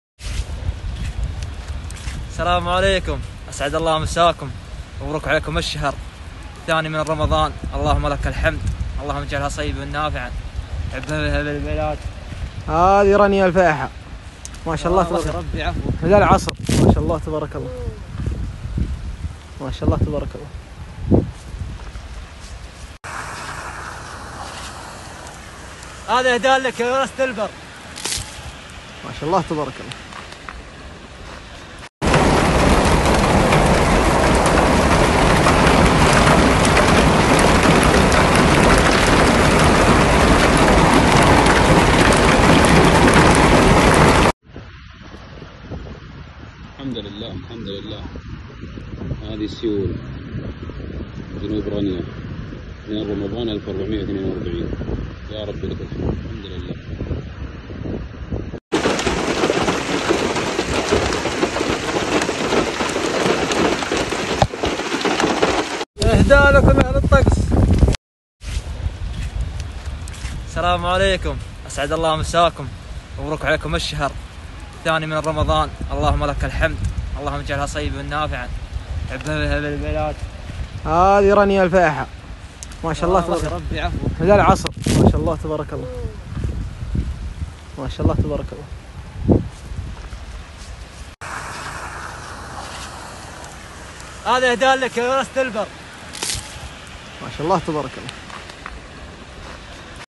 INNALILAHI❗️ARABSAUDI DIHUJANI BATU ES DAN SALJU
guys ini video kiriman dari teman saya,,,yuk simak video nya...semoga kita selalu ada dalam lindungan Allah,,,aamiiin.